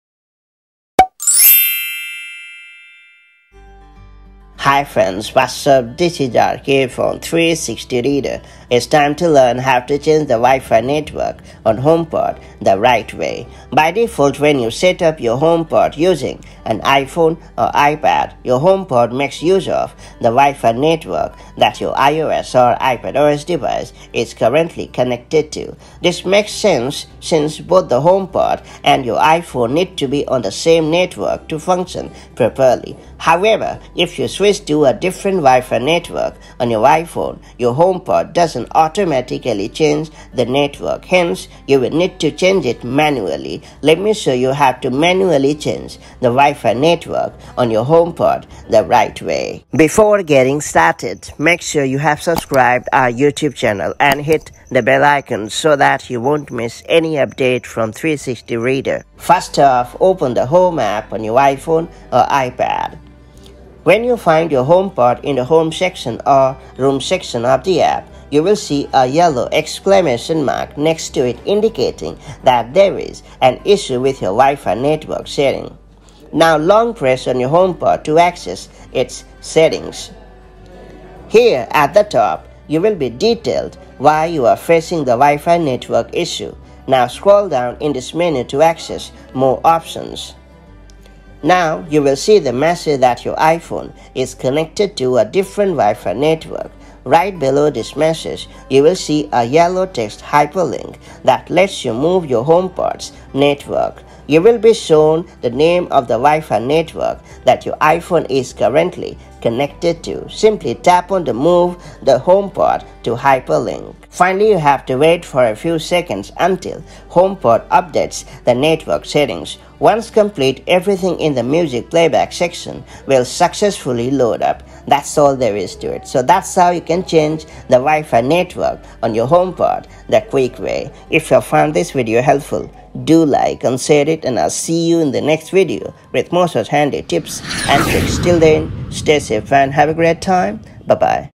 How To Change HomePod Wi-Fi Network
Need to change the Wi-Fi network for your HomePod? This guide will show you how to switch your HomePod to a different network quickly and effortlessly. Whether you're setting up a new router or troubleshooting connectivity issues, we’ll walk you through the process step by step.